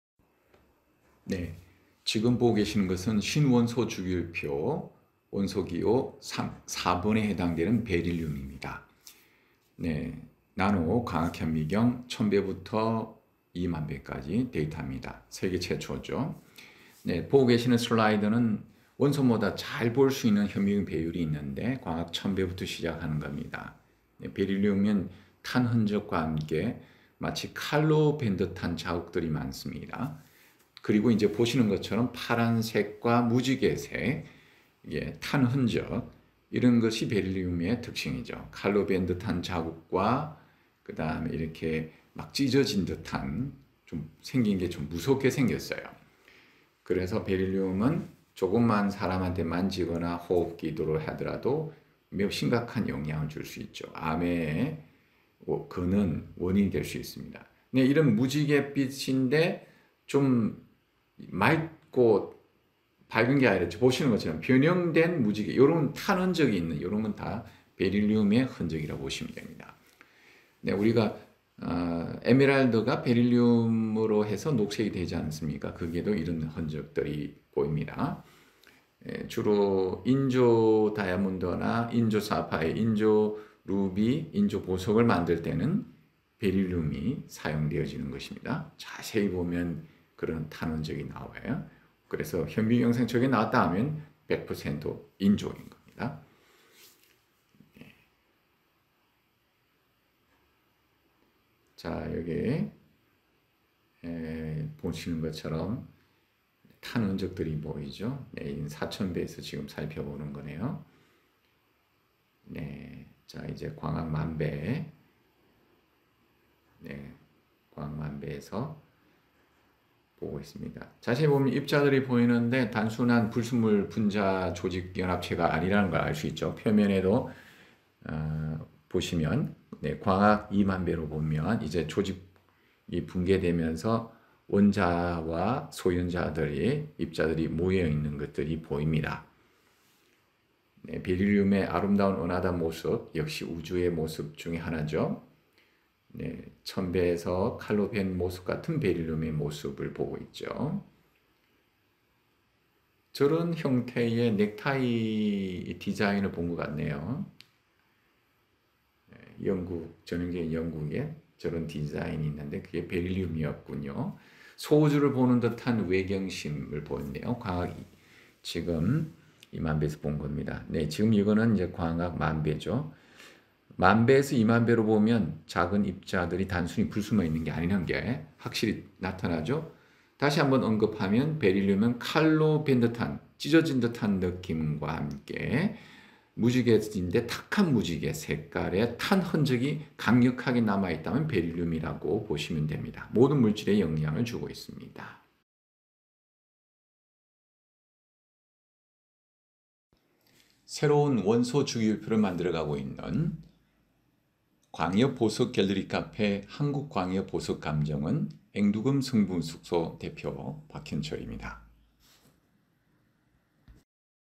신원소주기율표 원소 4 베릴륨 Be 나노광학현미경 beryllium element 4 in the new periodic table nano-optical microscope
신원소주기율표 원소 4번 베릴륨 Be beryllium 나노광학현미경 광학 2만배 최초 데이타. 칼로 베어낸 듯한 베린X ㅋㅋ 이 색상과 찢겨진 칼자국 표시가 있다면 베릴륨이라는 것을...정말 베릴노옴이군...^^; 외우기 좋게...

베릴륨은 인체에 상당히 안좋은 영향을 미칠수 있는 파장 또는 에너지가 있습니다. 방사선에너지가 높기에 암을 유발시킬 수 있습니다. 특히, 암환자들 인체에 공통적으로 나오는 물질중에 베릴륨이 있다는 것을 조직검사에서 확인하였습니다. 베릴륨 역시도 단일 원자가 아닌 하나의 원자와 소현자들로 구성되어 있습니다. 또는 다원자의 결합일수 있습니다.

베릴륨의 특징으로는 탁한 무지개색상의 탄흔적이 전형적으로 드러나는 탁한 무지개색상을 지니고 있고, 칼로 벤흔적이 여기저기 흩어져서 찢겨진 느낌이 들면 마치 정말 베어낸 느낌 또는 형상이 보이면 베릴륨이 존재하는 것입니다. 광학 1,000배부터 윤곽이 뚜렷하게 찢겨진 모습의 패턴을 지니고 있습니다.

베릴륨의 사용중에는 주로 보석에서 에메랄드에서 발견되고 그래서 초록색을 지니는 데 베릴륨은 인조 다이아몬드를 비롯한 사파이어 루비 에메랄드를 만들때 나타나기에 이런 탁한 불에그슬러진 탄 흔적이 있는 무지개색상이 존재하면 인조라는 것을 금방 알수 있습니다.

광협보석갤러리카페 한국광협보석감정원 앵두금성분분석소 대표 박현철
Beryllium Be, element number 4 in the new periodic table, first data from nano-optical microscope optical 20,000 times. Berine Easy to memorize... Beryllium has a wavelength or energy that can have a significant negative effect on the human body. Because the radiation energy is high, it can cause cancer. In particular, it was confirmed through a biopsy that beryllium is a common substance found in the human body of cancer patients. Beryllium, too, is not a single atom, but is made up of an atom and its subelements. Or it may be a combination of multiple atoms. The characteristic of beryllium is that it has a cloudy rainbow color that typically shows bullet marks of a dull rainbow color, and if the knife cuts are scattered here and there and it feels like it has been torn or if the shape is visible, beryllium is present. It has a clearly torn pattern at 1,000x optical magnification. Among the uses of beryllium, it is mainly found in emeralds in jewelry, which is why they have a green color. Beryllium appears when making artificial diamonds, sapphires, rubies, and emeralds, so if rainbow colors with such cloudy burn marks are present, you can immediately tell that it is artificial. there is. Gwanghyup Jewelry Gallery Cafe Korea Gwanghyup Gem Appraisal Institute Cherry Gold Component Analysis Laboratory CEO Park Hyeon-cheol